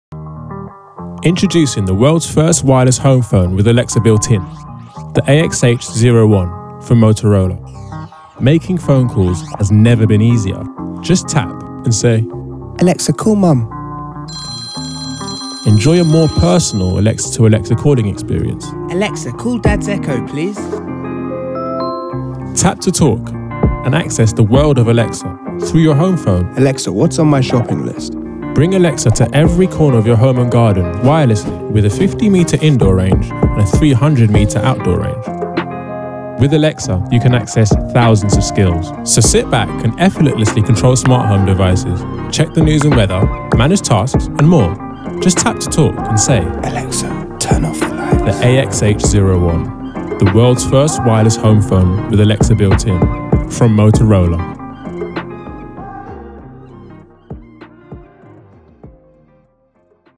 Motorola AX Series - Wireless Home Telephone with Alexa built-in
We are delighted to announce the launch of the Motorola AX Series with Alexa built-in. A real game changer for the Wireless Home Telephone - a product long considered a lifeline for many and responsible for connecting billions of conversations worldwide everyday - we introduce Alexa and voice recognition to a wider audience.